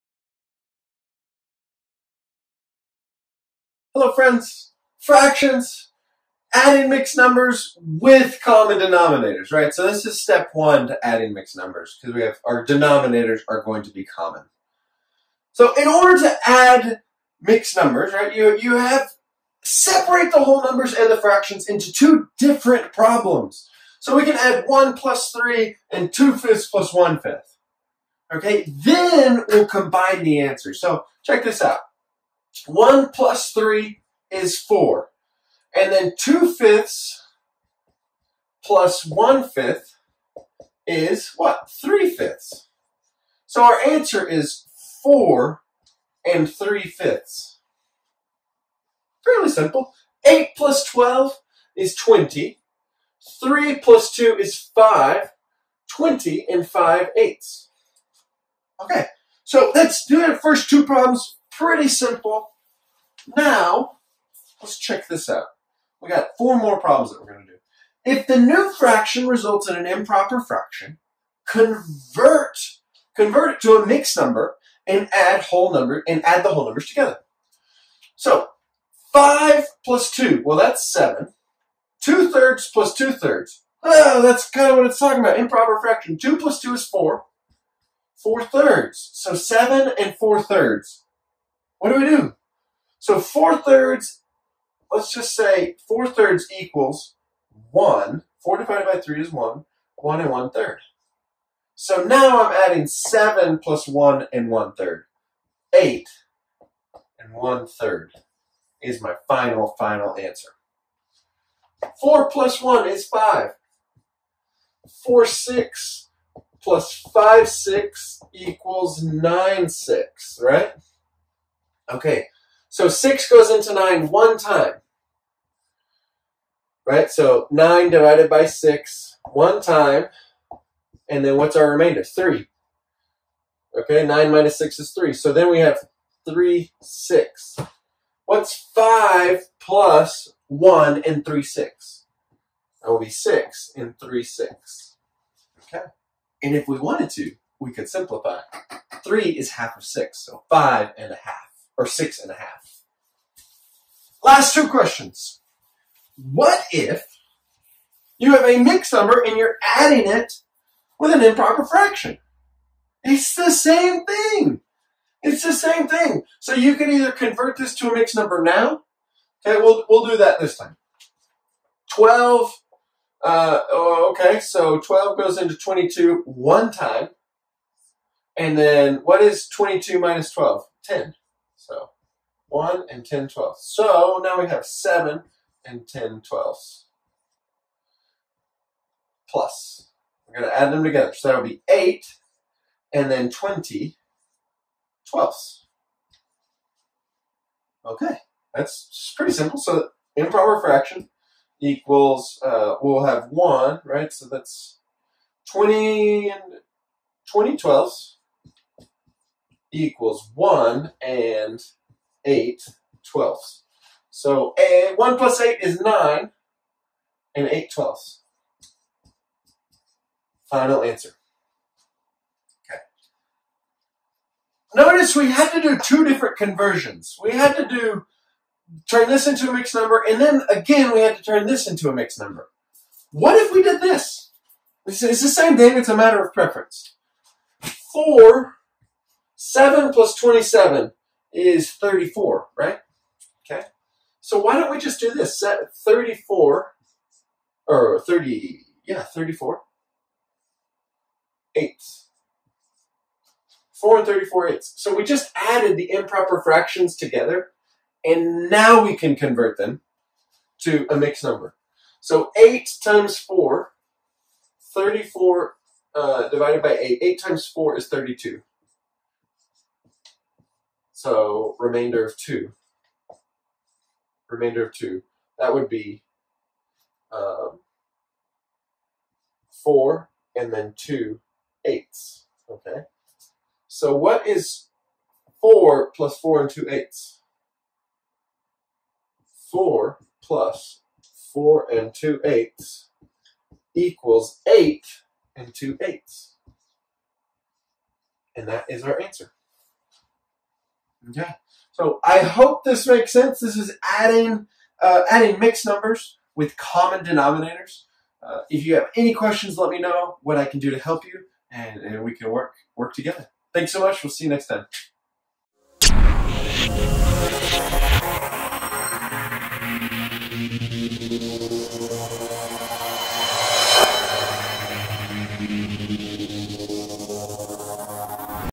Fractions - Adding Mixed Numbers with Common Denominator
Practice adding mixed numbers with common Denominators.